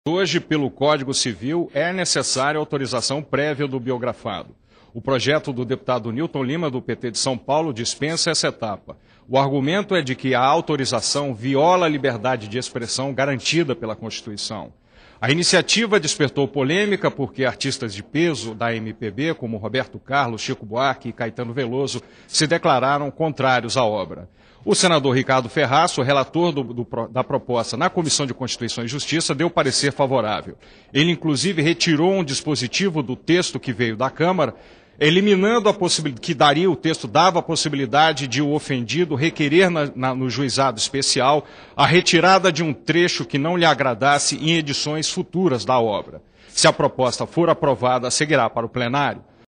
Liberação de biografias não autorizadas está na pauta da CCJ
A Comissão de Constituição e Justiça (CCJ) deve analisar nesta quarta (25) a proposta que altera o Código Civil para liberar a publicação de biografias não autorizadas de pessoas públicas. O relator da matéria, senador Ricardo Ferraço (PMDB-ES), já apresentou parecer favorável pela aprovação. Veja mais informação com o repórter Antonio Machado, da TV Senado.